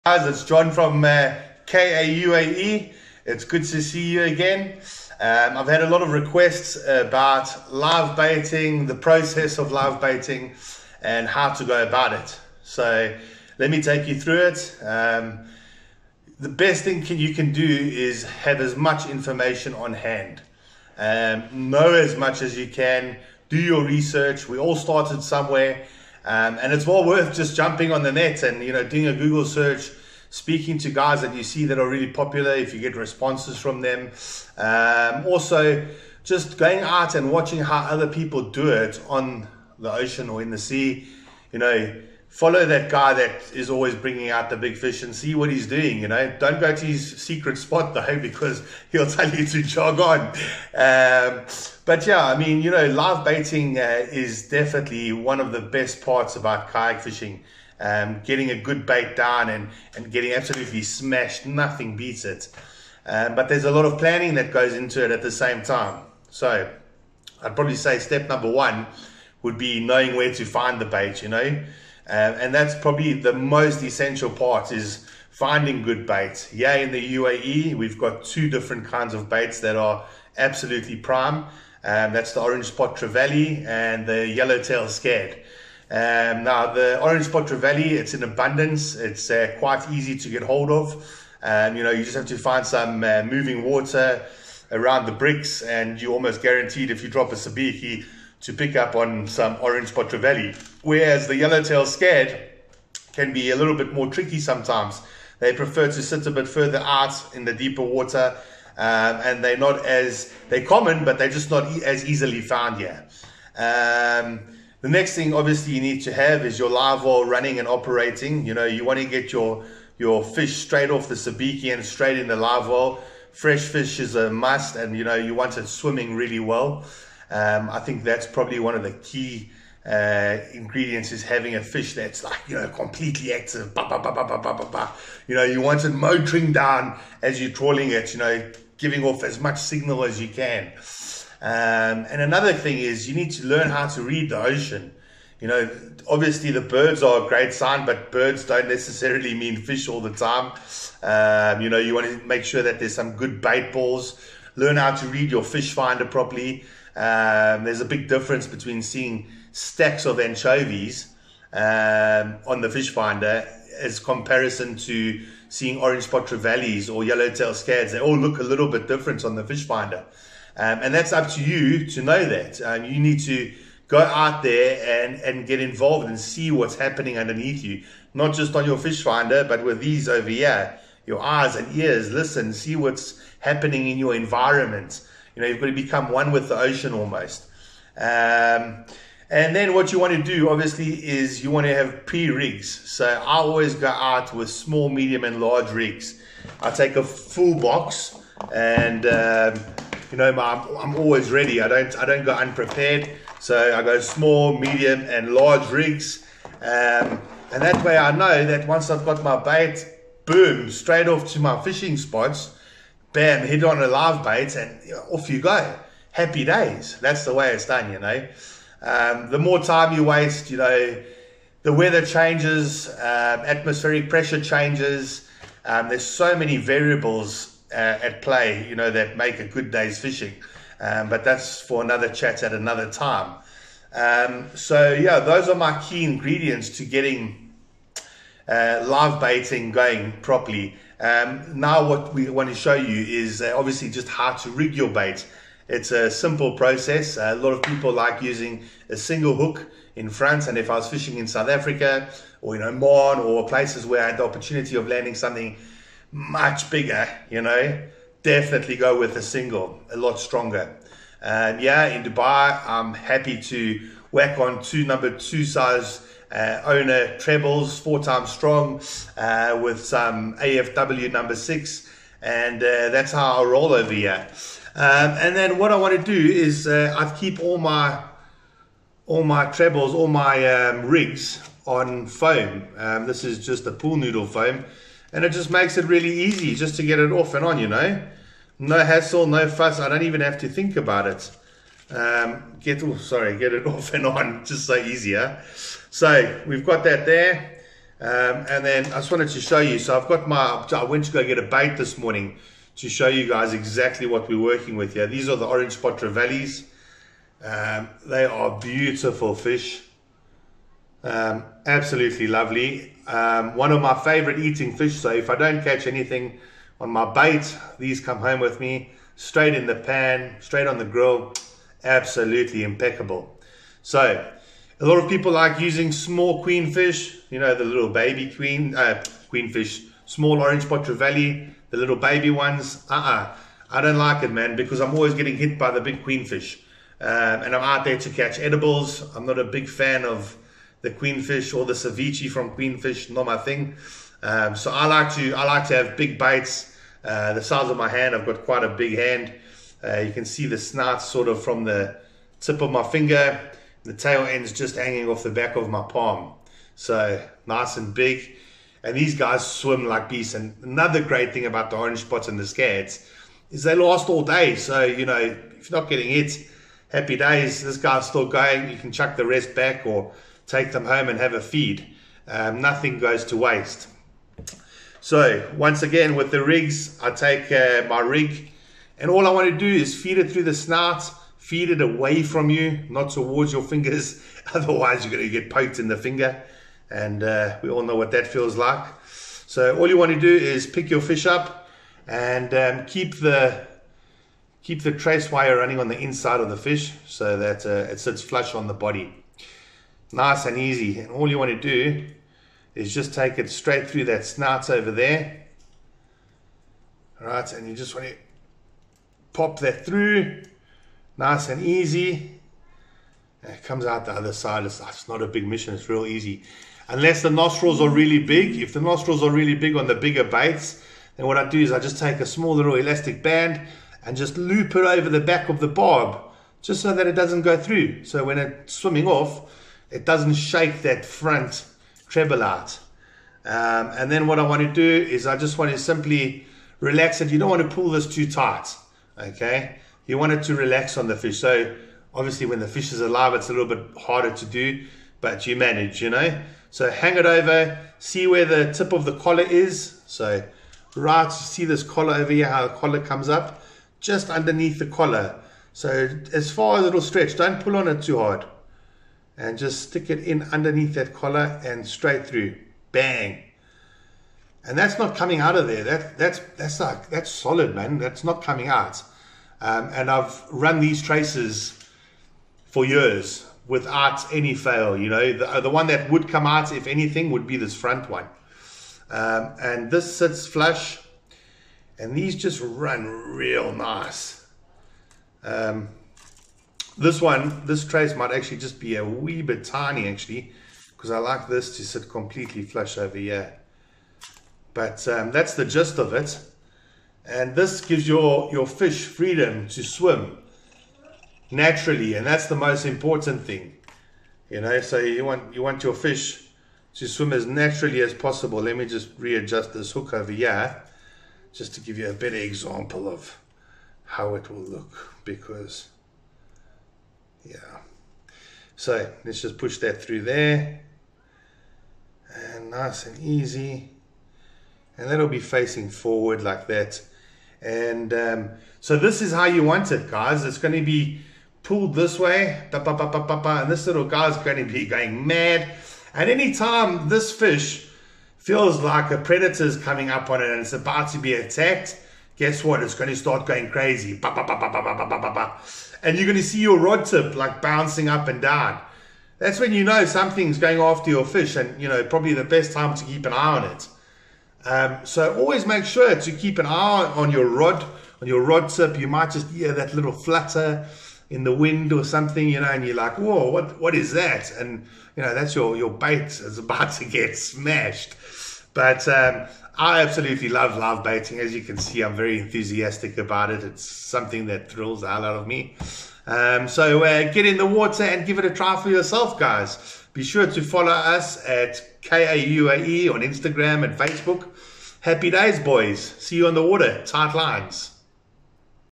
Live Baiting Tips / Mackerel -Dubai
We all want to get smashed by that big Mackerel!

Live baiting definitely helps put you on the right path, there simply is no better bait than a live bait on the end of your line!

When that bait starts twitching you know it's going down!

Get on it boys! Time to get Tight🔥🔥🔥